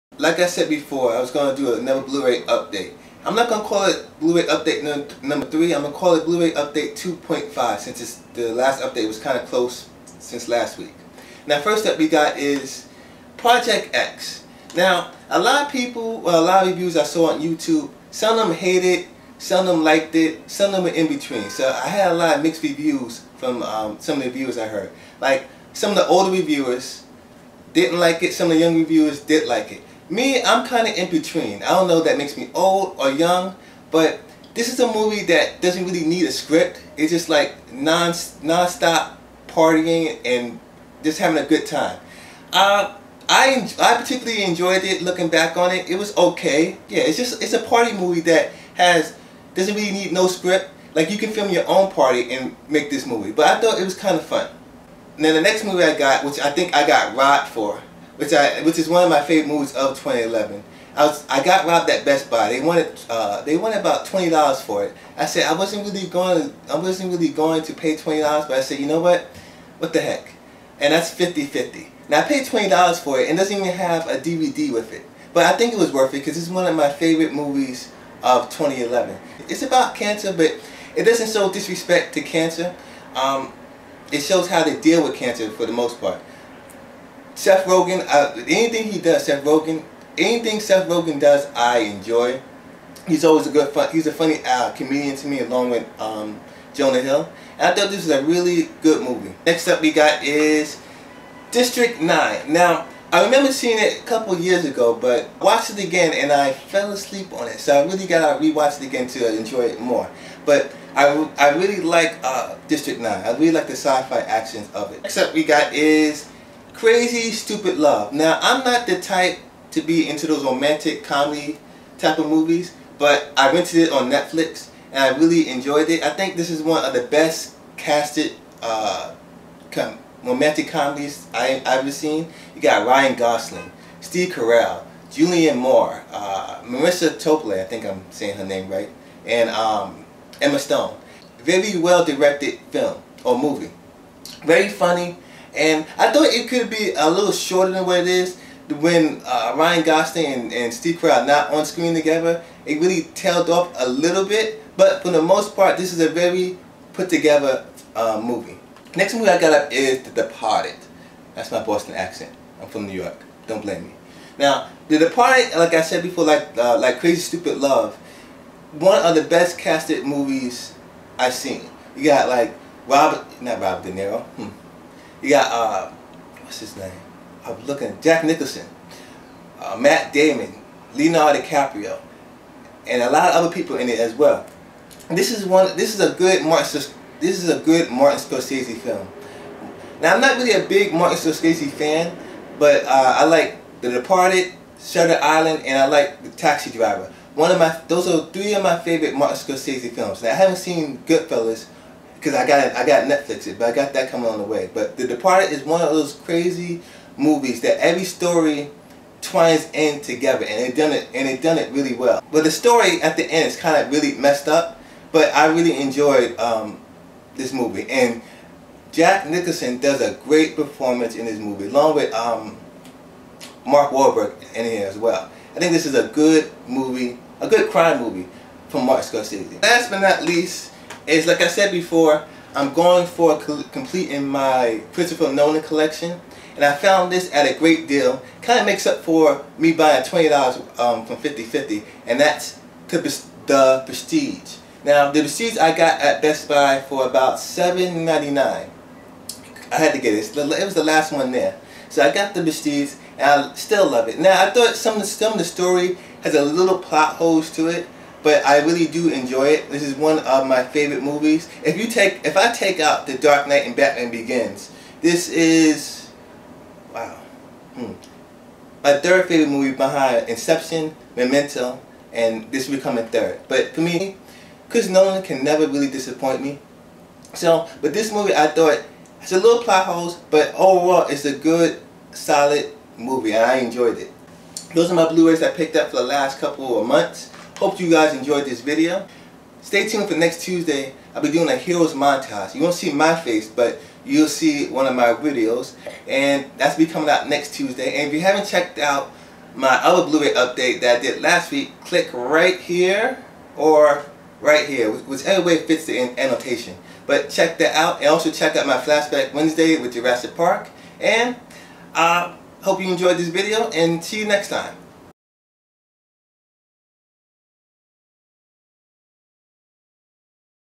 Blu Ray Update 2.5
Part 2 of my Blu Ray movies I picked Up In the last few months.